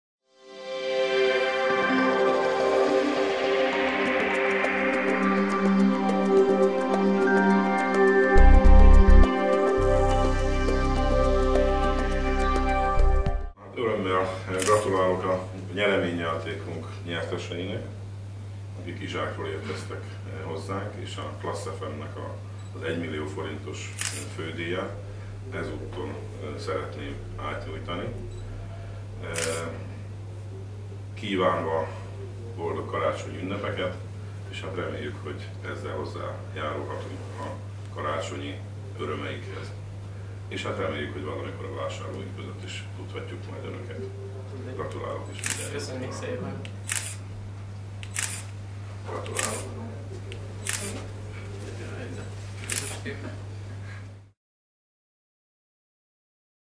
MARSO 1.000.000Ft-os nyeremény átadása (ClassFM)
A Class FM "Zene 4 keréken" játékában kisorsolt, a MARSO által felajánlott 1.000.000Ft-os nyeremény átadása.